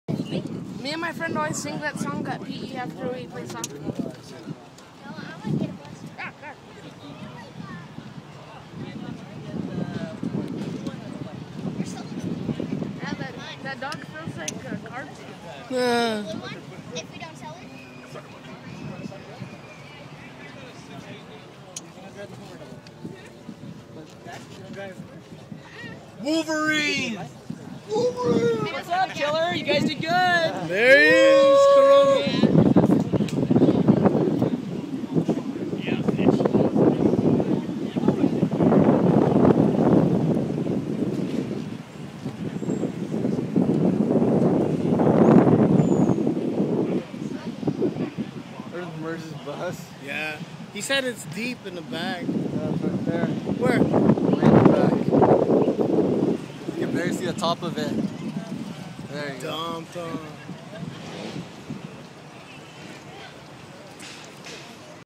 OEBC Spring Picnic 2010
Cruising through the grounds right after 10ft Doug cleaned up the slow drags